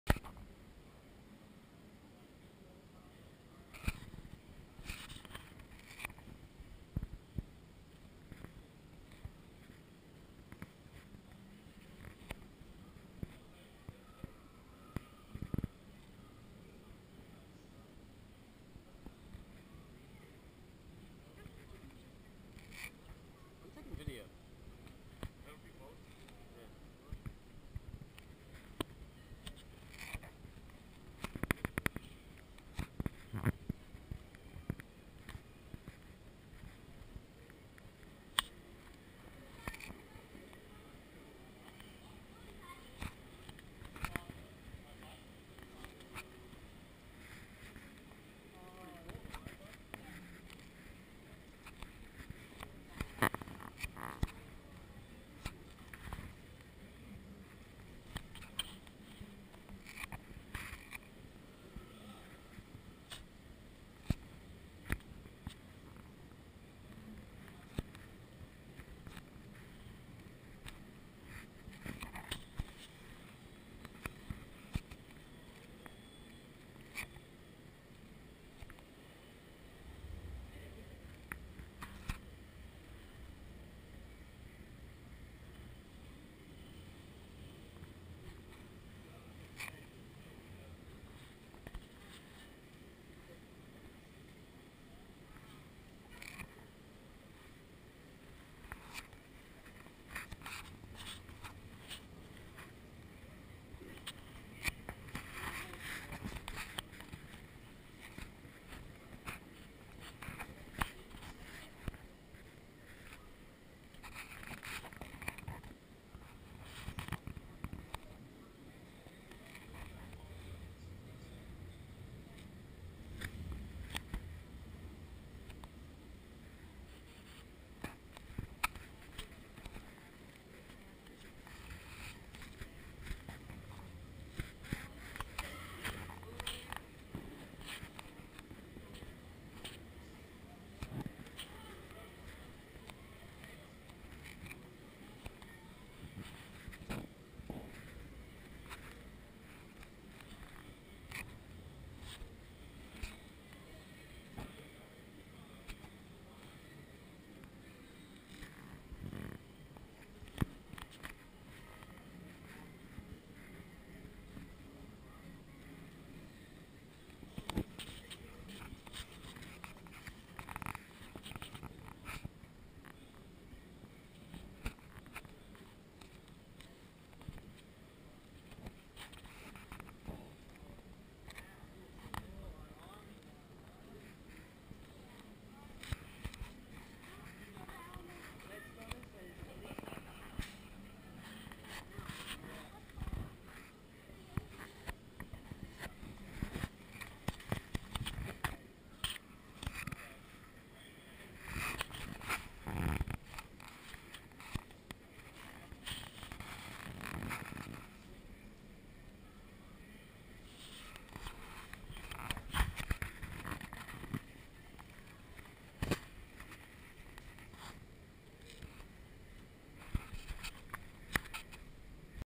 Inside Look at the Timeline Exhibit at the H-D® Museum
Inside Look at the Timeline Exhibit at the H-D® Museum in Milwaukee, WI. Video courtesy of Rocky.